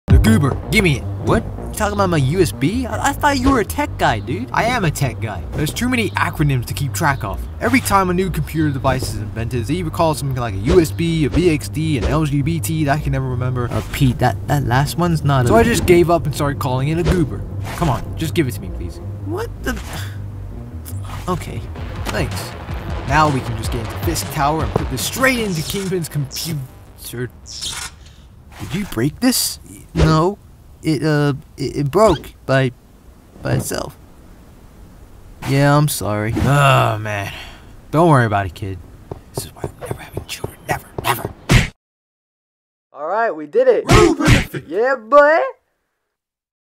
Spider-Man and the Goober!
In this LEGO Video, Spider-Man wants his Goober back from Miles Morales!

Based on the scene from "Spider-Man:Into the Spider-Verse" (2018), I wanted to do a LEGO Recreation when Miles meets Peter B.Parker for the first time, in the style and context of my LEGO Spider-Man x Miles Morales series as a Short Spider-Man video. I did add some minor effects like the purplish colour grading to give off that similar Spider-Verse vibe if you noticed. Man, I have to make more LEGO Spider-Verse videos bro haha.

I thought of this video idea for a while and there is a lil joke which some may or may not like but I don't think it's offensive. I personally think recently in these shorts, LEGO Peter Parker is a bit of a boomer similar to Peter B.Parker in a way. I think I could do something with that lol. Anyway lil upfate w/ Spider-Man SEASON FINALE! On Scene three starting the next fight scene soon and will be done Decemeber time come Christmas.

Music Used

Links